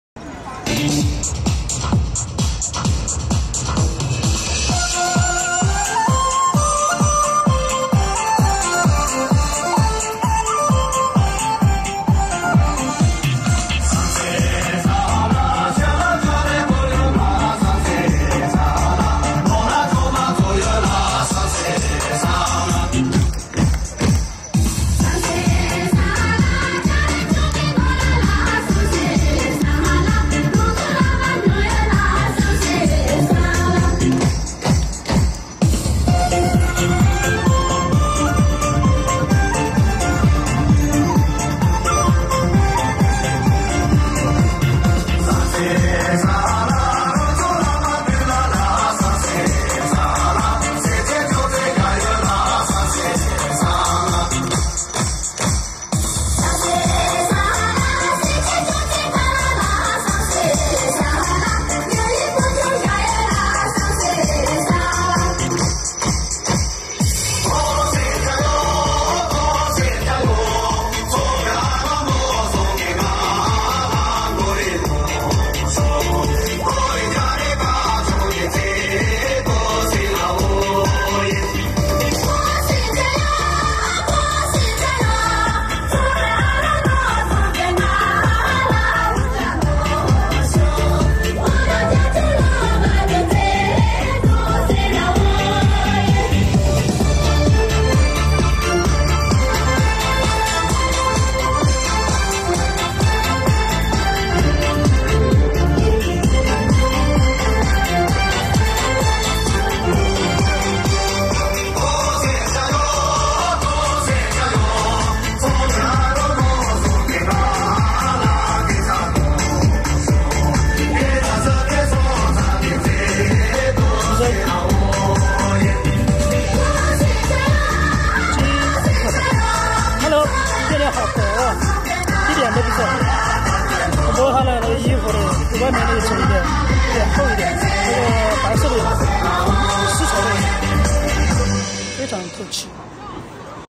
Yongji Zhuo Rechamu - A Tibetan Song and a Guozhuang Dance/雍吉卓熱查姆-一首藏歌一段鍋莊舞-整齊劃一的舞姿雍吉帶領大家一起鍋莊舞起來
Yongji Zhuo Rechamu - A Tibetan Song and a Guozhuang Dance
雍吉卓熱查姆-一首藏歌一段鍋莊舞-整齊劃一的舞姿雍吉帶領大家一起鍋莊舞起來
視頻來源於2023年4月19日
星期三正裝之日
☁️晚上天氣30度
很多隊員沒有穿隊服，主要還是太熱了。成都市連續幾天刷新同期歷史記錄。天氣涼快一些肯定會統一穿上新隊服。
週三晚上雍吉來得比較晚，過了四十分鐘才來，衣服也不是隊服，是綠色的藏裝。

📷📸Camera+Lens+Gimbal
📹📷📸🎥【My Gear】
➠Lens:SONY
➠Gimbal: DJI RS3+DJI RS3 Mini
🌏From Chengdu,China.